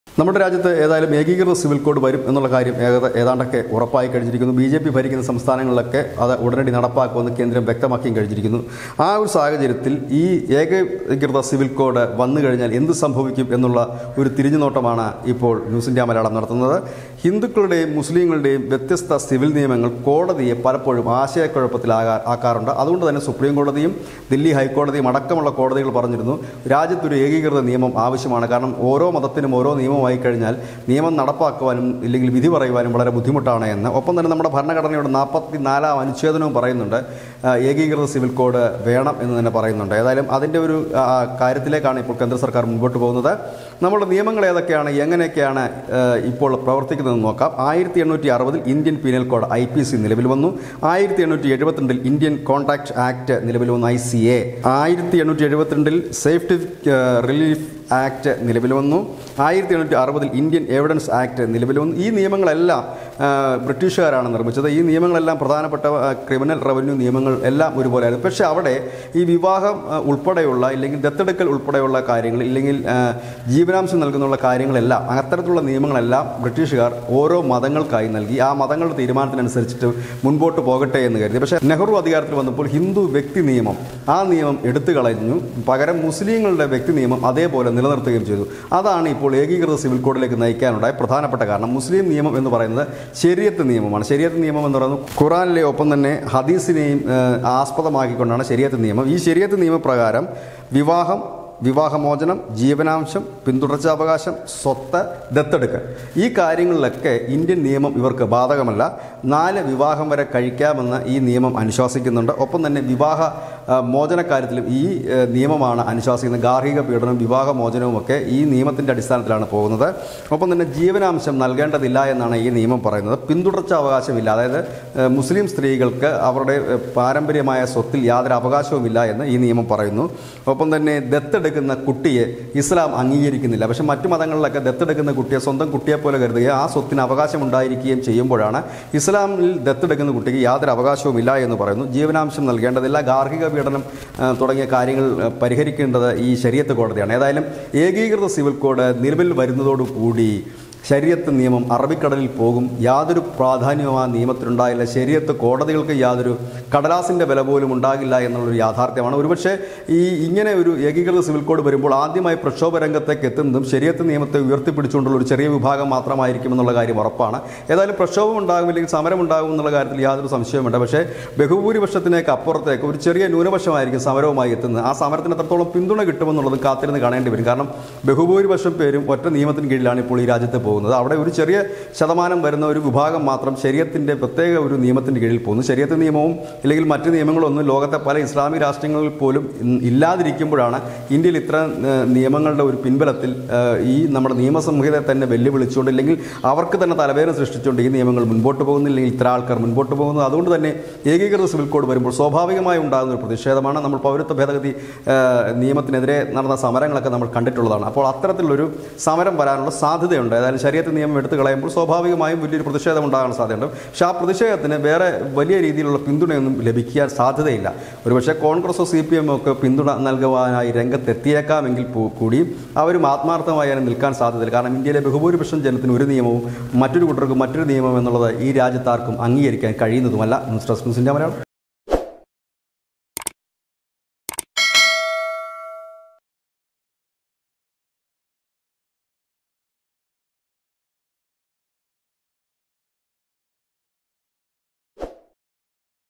പ്രക്ഷോഭത്തിന് സാദ്ധ്യത | Uniform Civil Code | NEWS INDIA MALAYALAM POINT
ഏകീകൃത സിവില്‍കോഡ് വന്നാല്‍ സംഭവിക്കുക ഇതാണ്!!; പ്രക്ഷോഭത്തിന് സാദ്ധ്യത